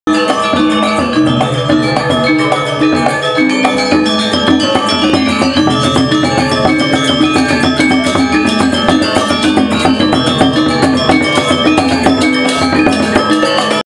Gamelan at the Ceremony of Barong in Padangbai, Bali
The percussion instruments of the gamelan enchanting the crowds whilst waiting for the beginning of the ceremony.